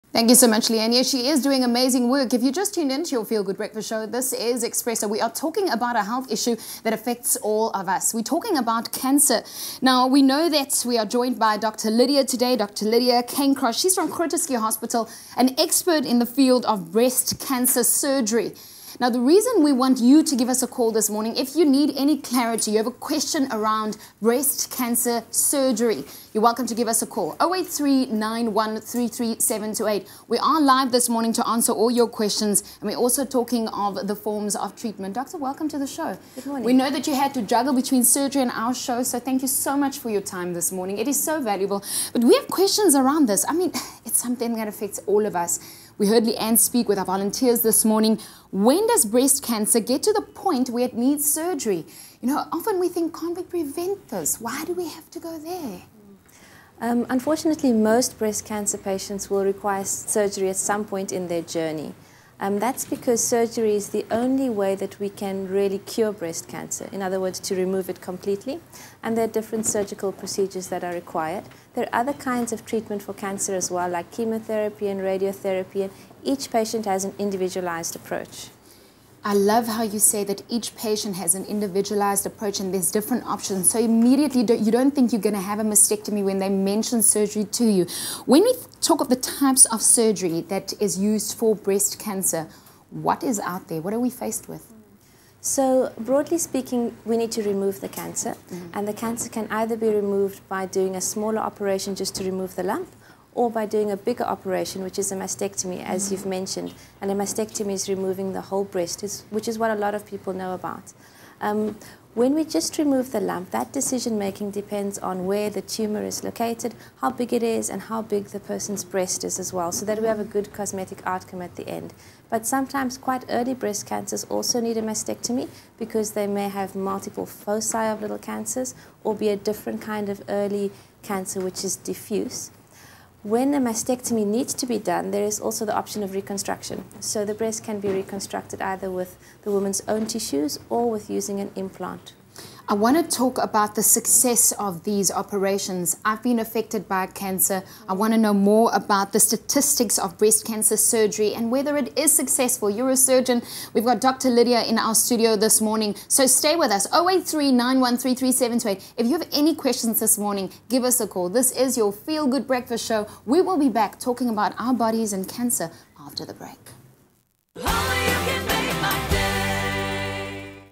Understanding Breast Cancer Surgery - Dr Lydia Cairncross - Part 1
For today’s health segment we chat to Dr Lydia Cairncross from Groote Schuur hospital, who is an expert on breast cancer surgery, to hopefully get a better understanding of what goes into this very important form of treatment.